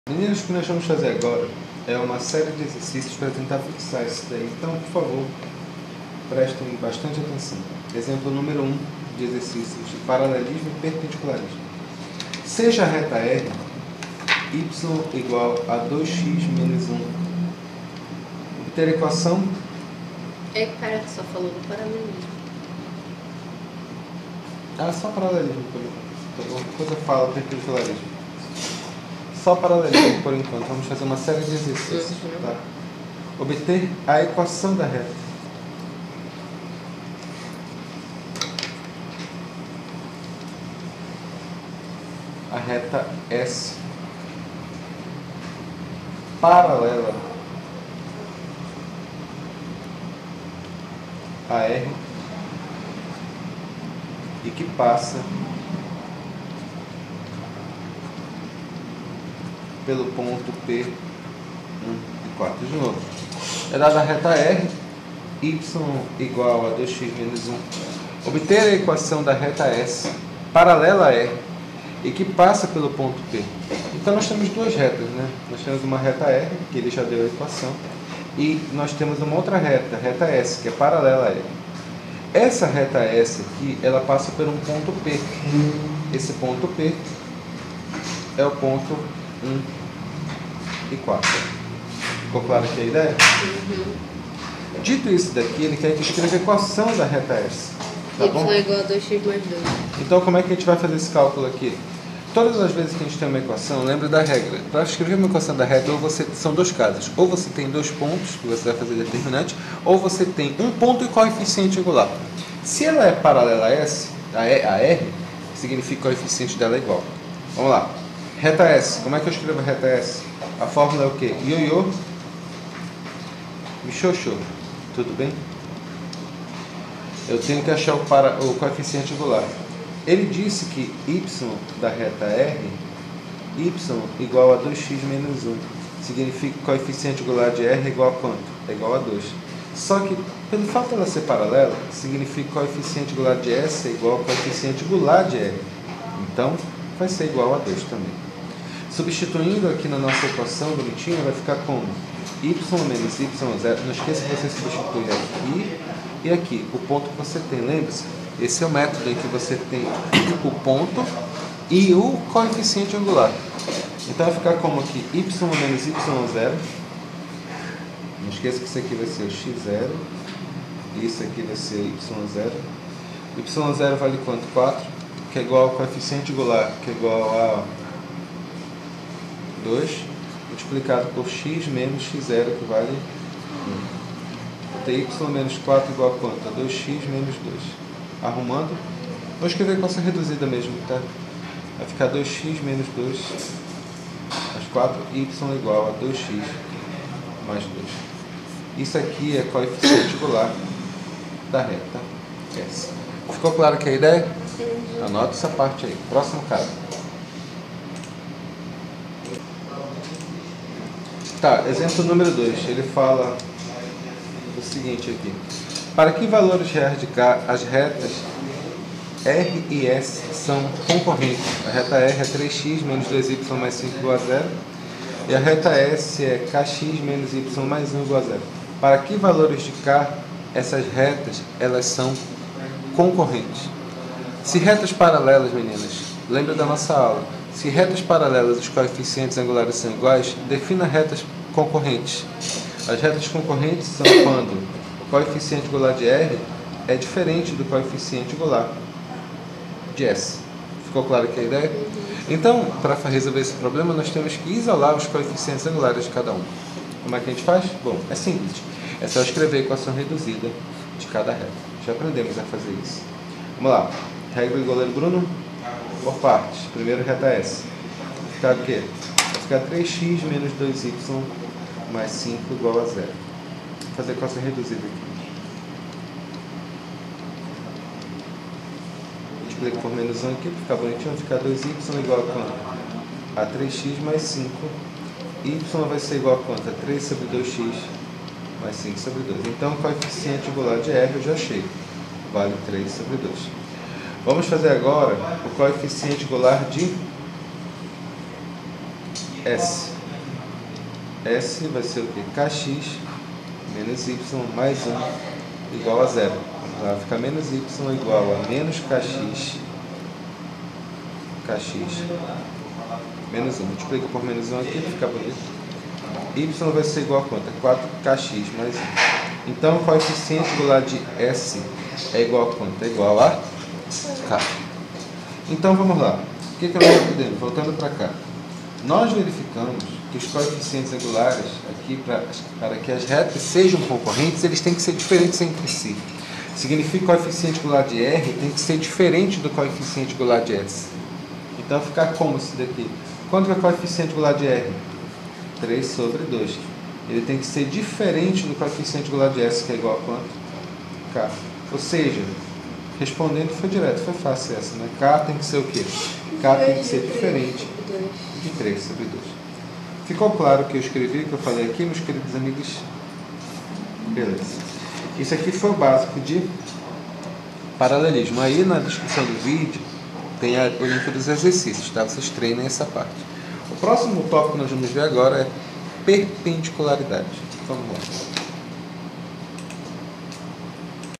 Paralelismo entre Retas - Parte 2 (Exercícios)
- Classificação e escrita de uma reta em função de um ponto.
- Equação da reta: Aplicação e Exercícios.
- Como escrever uma equação paralela a partir de um ponto: Temos um ponto e o Coeficiente Angular.